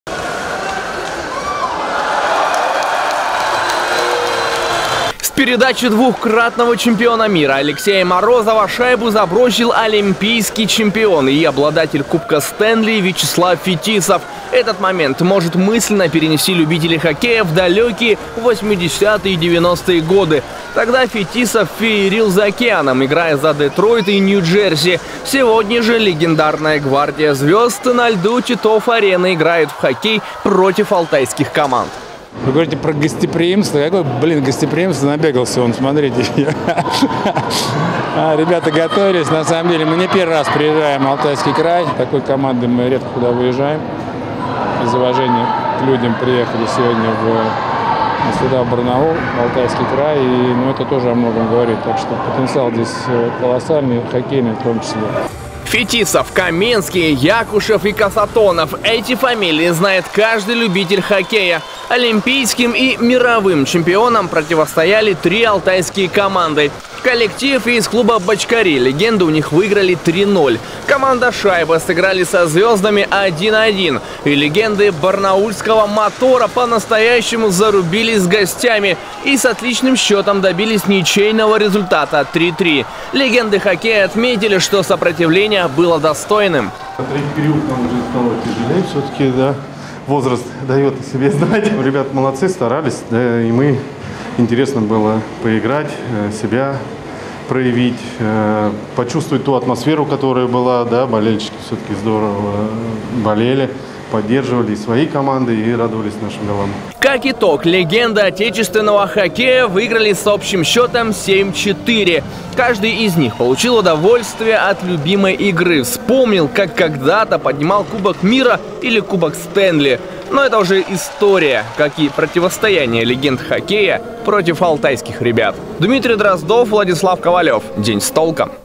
Легенды хоккея провели гала-матч на льду "Титов-Арены" в Барнауле
После общения с болельщиками и автограф-сессии, легенды взяли в руки клюшки и провели гала-матч. На льду "Титов-Арены" звезды встретились с тремя Алтайскими командами. Кто противостоял чемпионам мира и обладателям Кубка Стэнли?